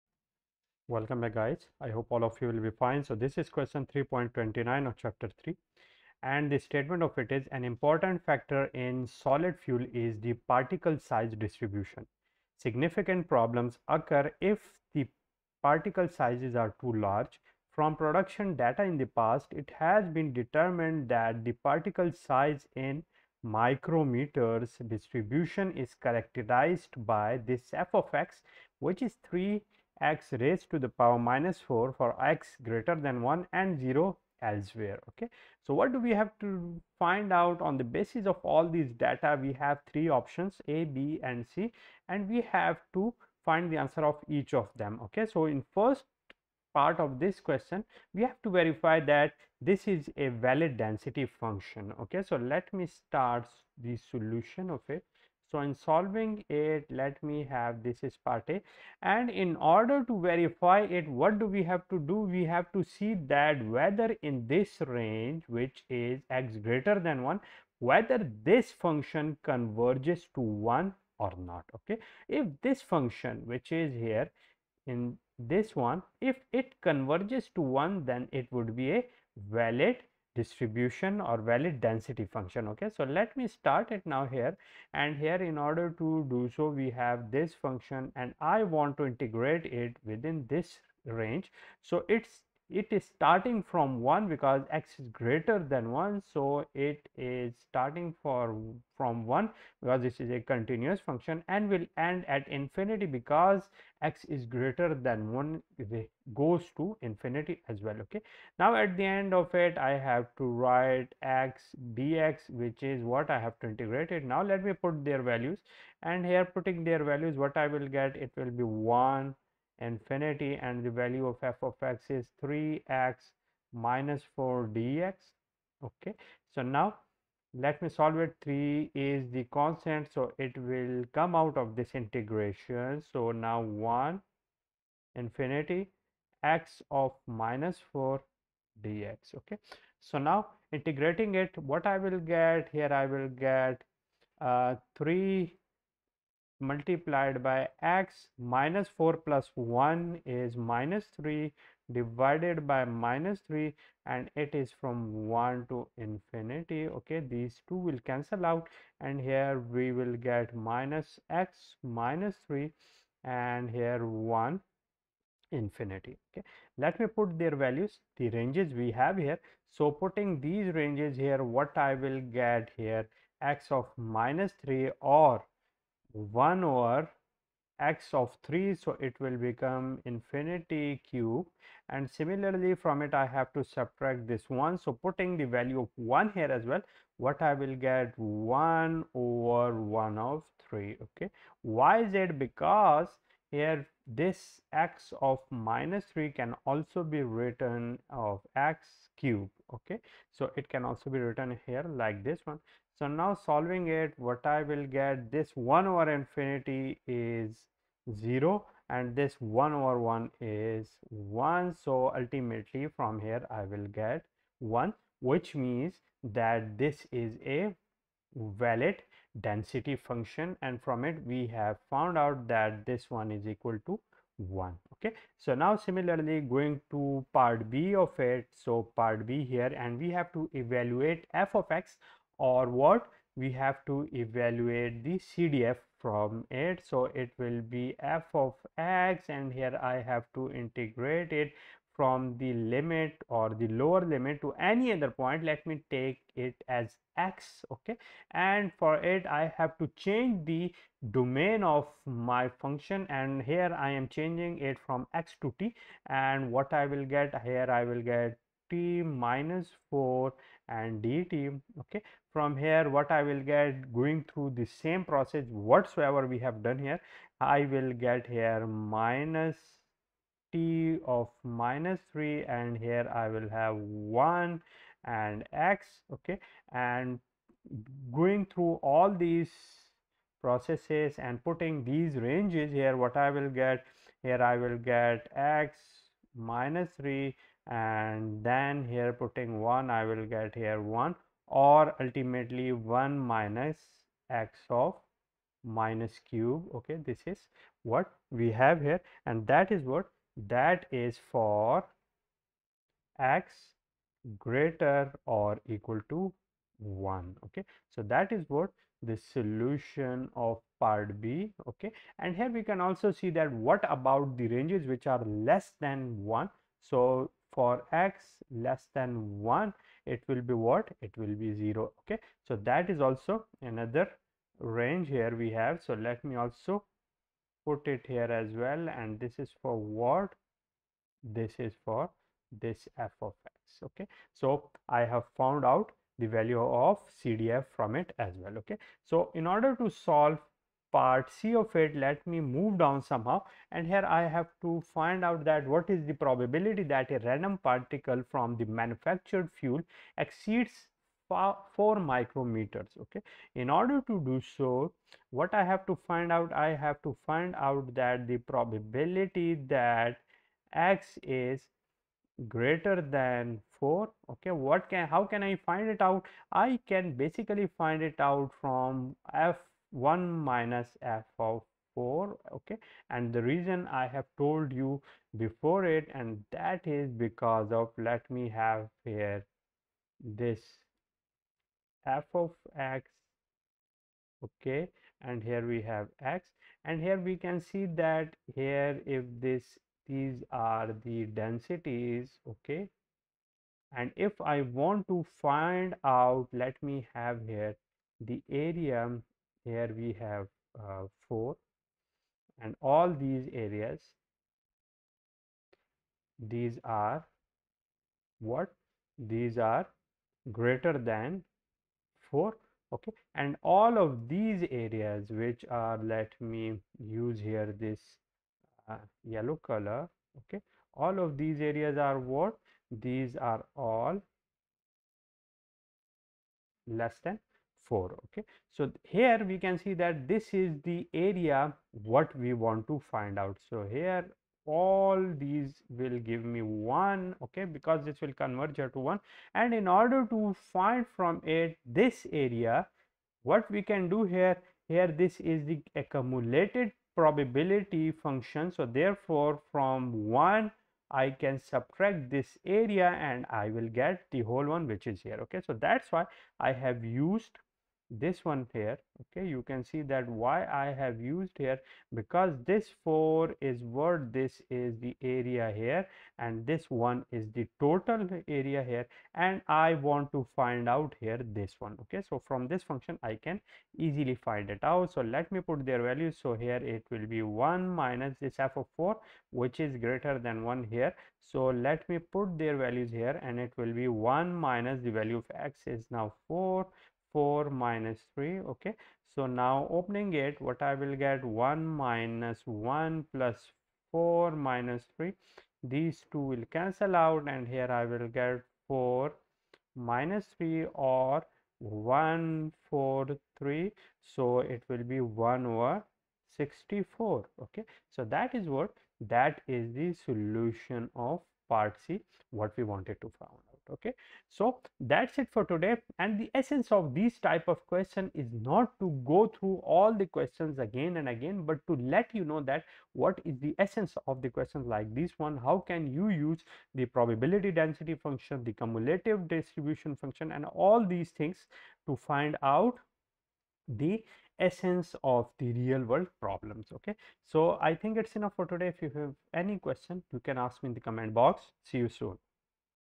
Solution Continuous Random Variable | Probability & Statistics for Engineers & Scientists by Walpole
This video describes the conceptual overview related to continuous PDF and its usage in the form of problem 3.29 to calculate different ranges of continuous probabilities.  In this problem, I showed how you can find probabilities from the given ranges of density functions.

3.29 An important factor in solid missile fuel is the particle size distribution. Significant problems occur if the particle sizes are too large. From production data in the past, it has been determined that the particle size (in micrometers) distribution is characterized by
f(x) = 3x−4, x greater than 1,
0, elsewhere.
(a) Verify that this is a valid density function.
(b) Evaluate F(x).
(c) What is the probability that a random particle from the manufactured fuel exceeds 4 micrometers?

Happy Learning :-)